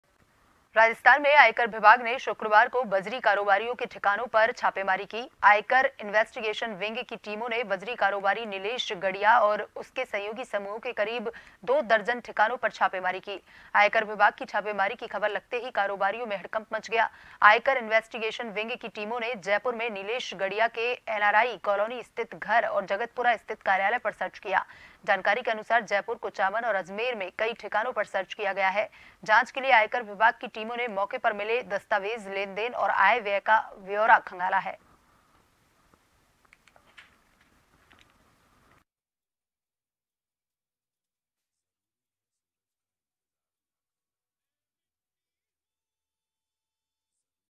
Income Tax Raid | राजस्थान के बजरी कारोबारियो के कई ठिकानों पर छापेमारी, कारोबारियों में मचा हड़कंप
Watch JAN TV on :
Tata Play DTH : 1185
Airtel DTH: 355
JIO Fiber: 1384